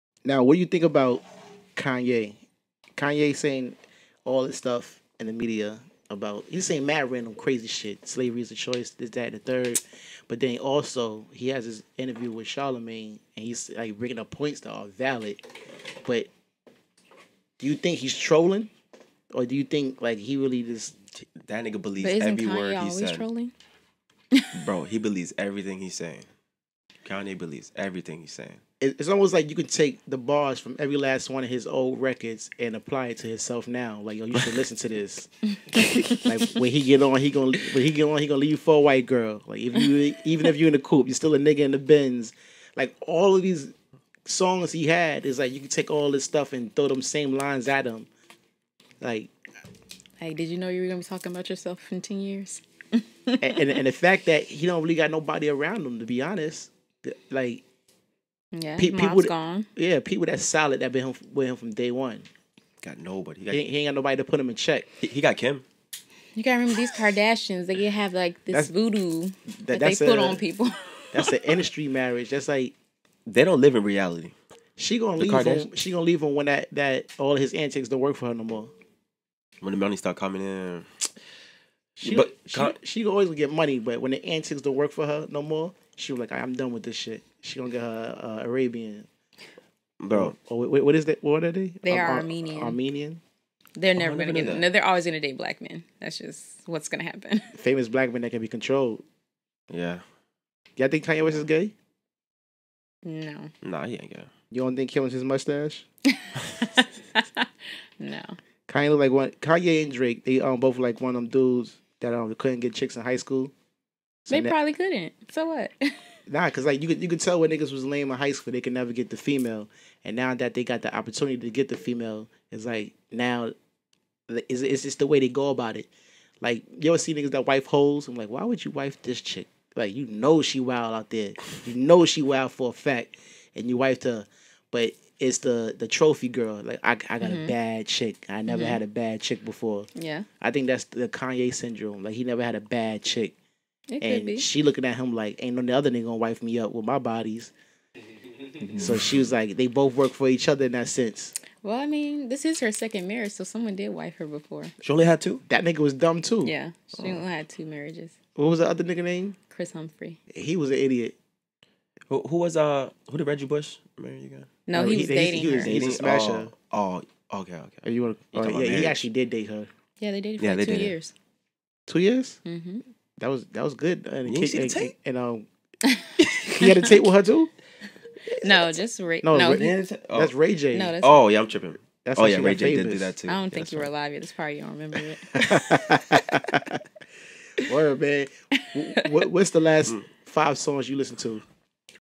Has Kanye lost his mind?
The Lite Nation Podcast discus their thoughts on Kanye's recent antics.